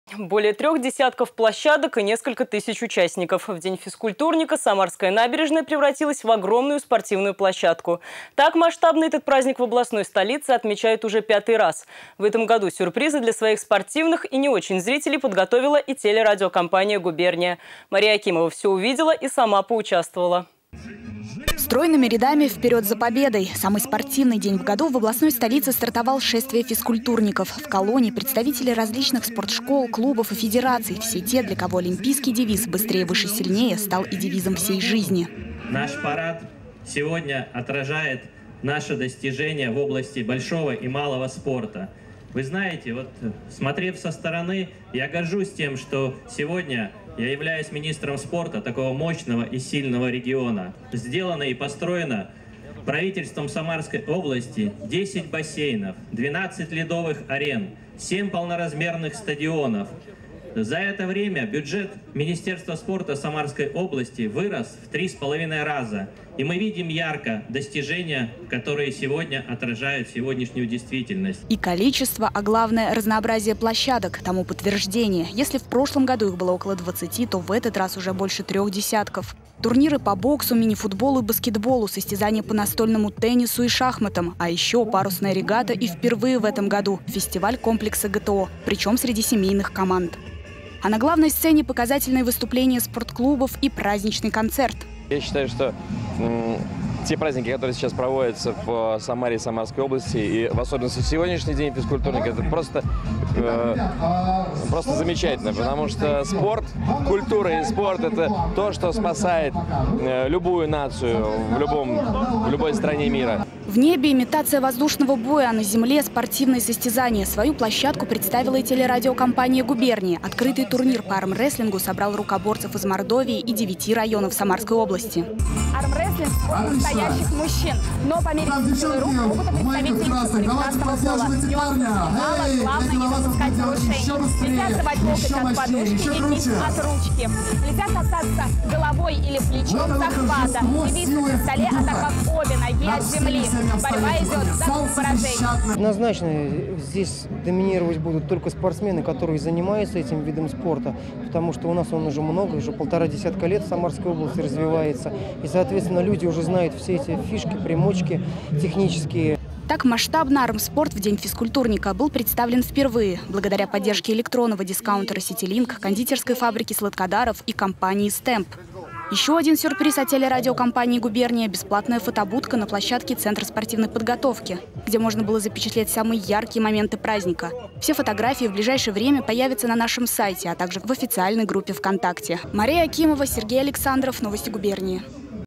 В Самаре масштабно отметили День физкультурника
Более трех десятков площадок и несколько тысяч участников: В День физкультурника самарская набережная превратилась в огромную спортивную площадку. Так масштабно этот праздник в областной столице отмечают уже 5-й раз. В этом году сюрпризы для своих спортивных и не очень зрителей подготовила и телерадиокомпания "ГУБЕРНИЯ". Мария Акимова все увидела и сама поучаствовала.